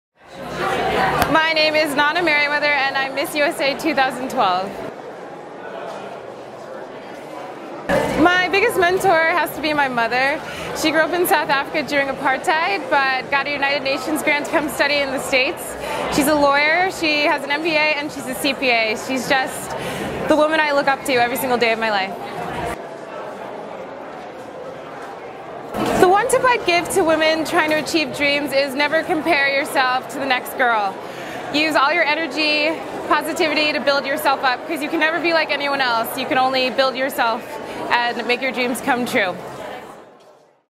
Nana Meriwether for Carrie Hammer's Role Models not Runway Models
Miss USA 2012 describes how female mentors have impacted her life and career as part of Carrie Hammer's Role Models not Runway Models fashion show at New York Fashion Week. Carrie selected real, inspiring women to walk the runway instead of typical high fashion models. Nana's biggest mentor is her mother, who grew up in South Africa during apartheid but gained a United Nations grant to study in the United States.

He advice for women looking to build a successful career is to never compare yourself to the next girl. Focus on building yourself instead of comparing to others and be your own inspiration to succeed.

Check out the popexpert workshop Role Models not Runway Models